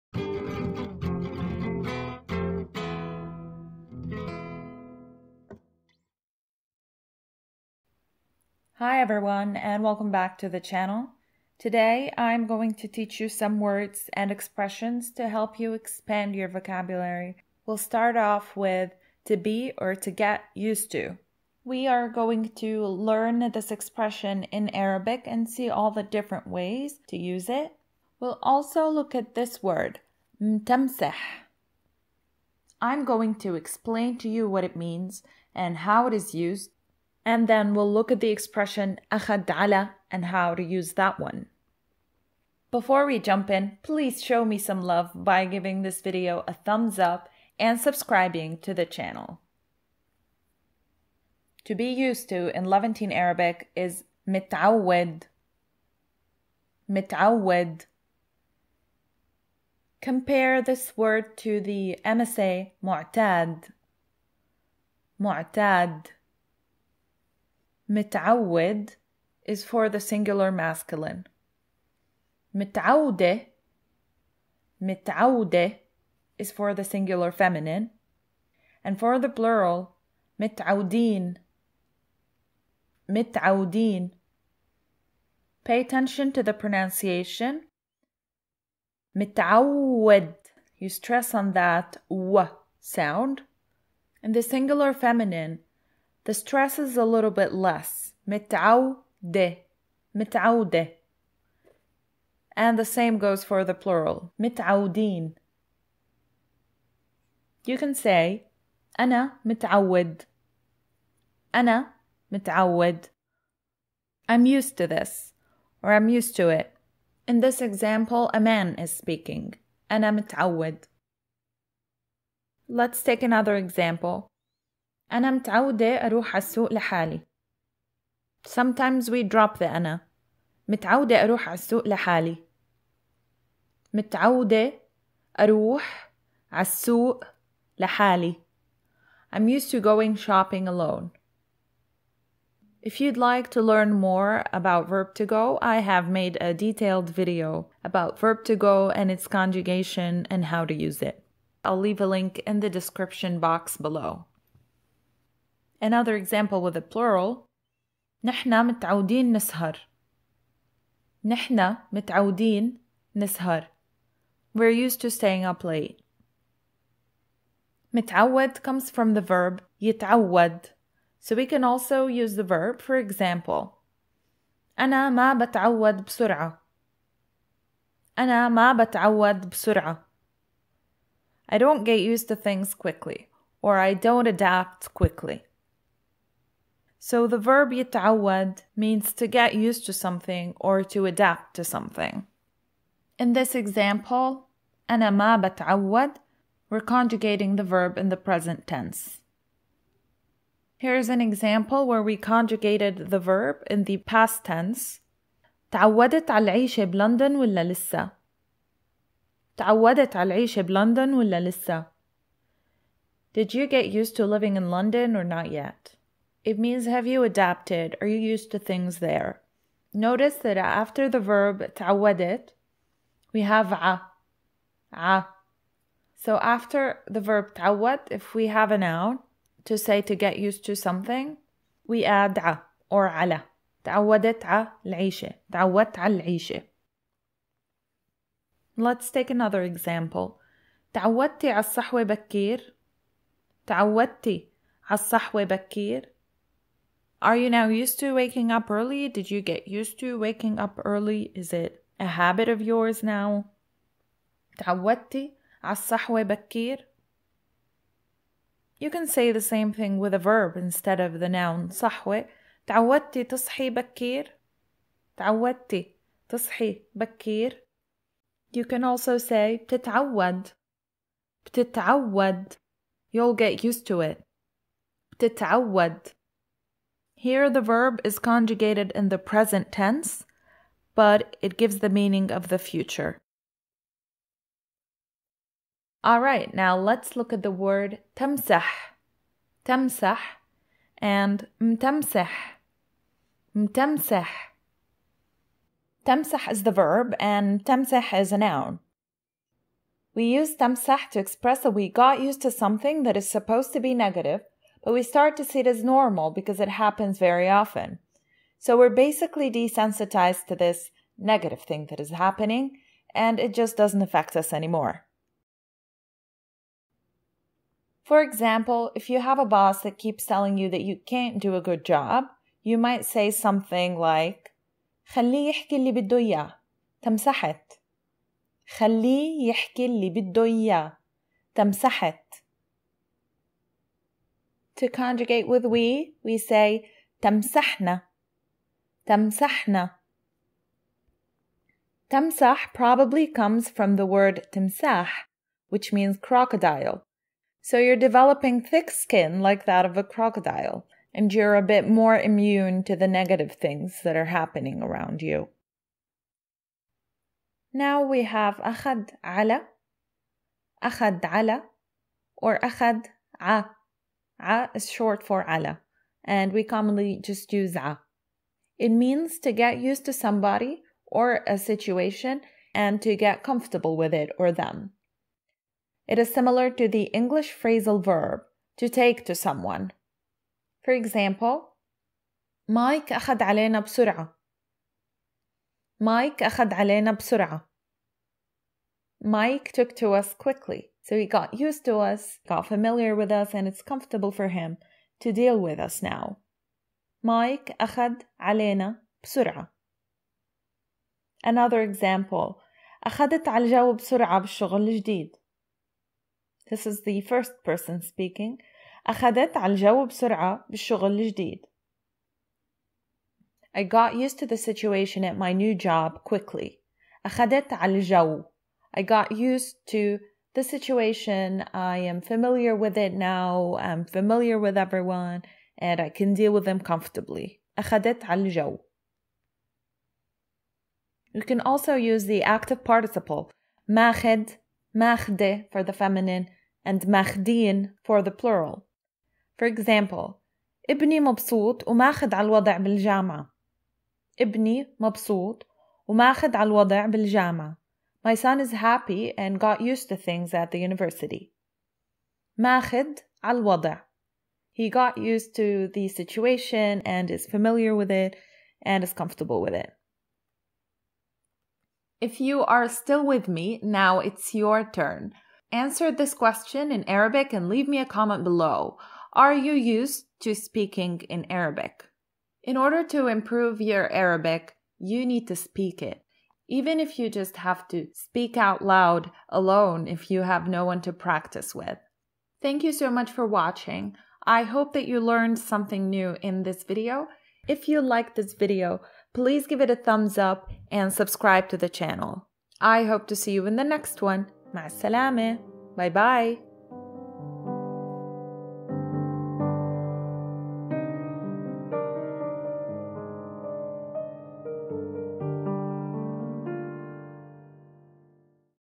Learn Levantine Arabic - How to say "I'm used to", "get used to", to adapt in Arabic
I've had a bit of a hard time explaining the sentences. It wasn't easy to exactly capture the meaning in English, but I hope that I made it clear enough.

I think these expressions don't translate literally into English but you could still convey the meaning. But that's what makes learning these expressions worthwhile. They're very useful in conversation and make you more fluent when you use them correctly.

SHOP AMAZON AND SUPPORT LEARN LEVANTINE ARABIC!